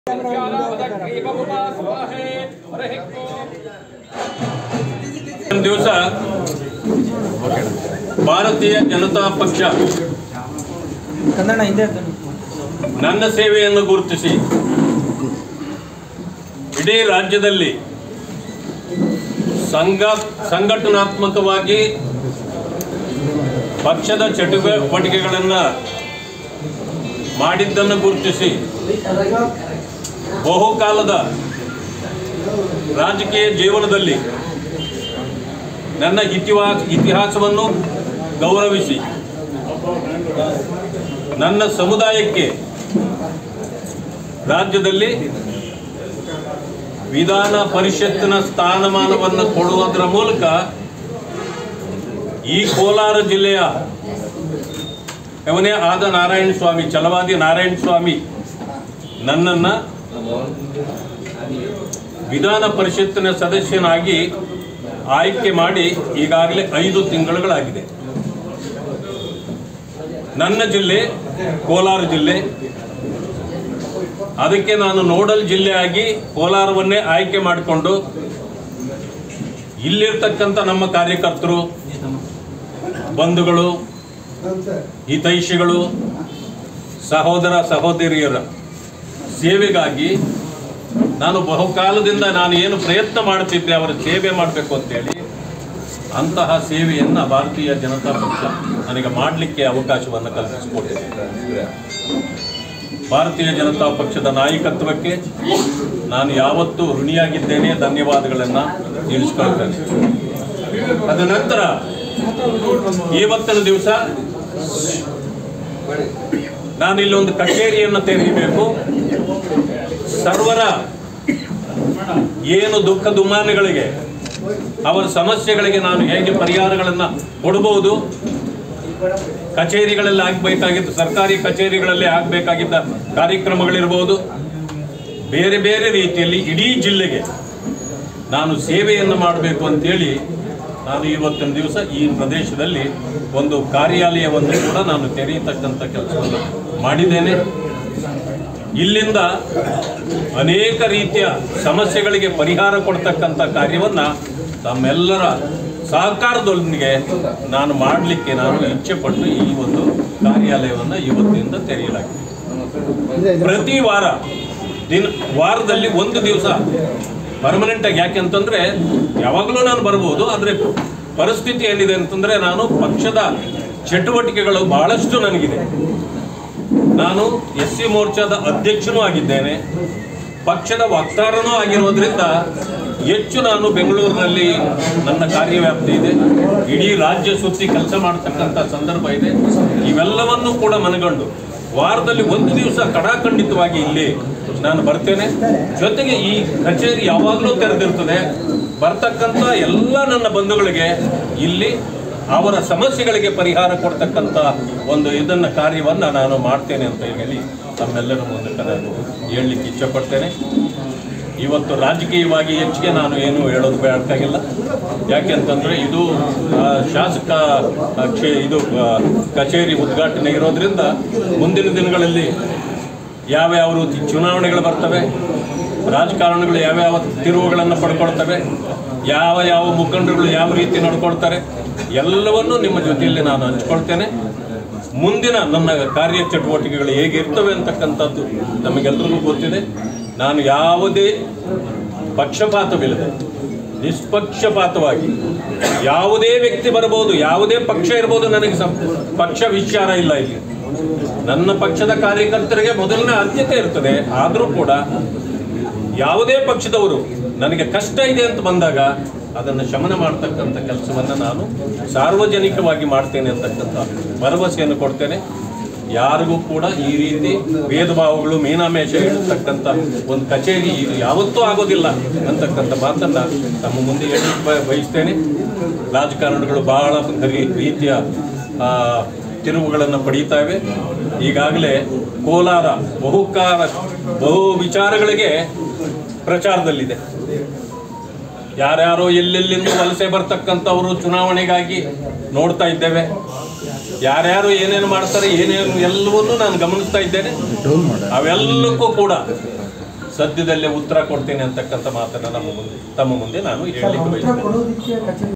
BJP MLC Chalavadi : ಕೋಲಾರದಲ್ಲಿ ನೂತನ ಆಡಳಿತ ಕಚೇರಿಯನ್ನು ತೆರೆದ MLC ಛಲವಾದಿ ನಾರಯಣಸ್ವಾಮಿ.|The Kolar News
ಕೋಲಾರದಲ್ಲಿ ನೂತನ ಆಡಳಿತ ಕಚೇರಿಯನ್ನು ತೆರೆದ MLC ಛಲವಾದಿ ನಾರಯಣಸ್ವಾಮಿ.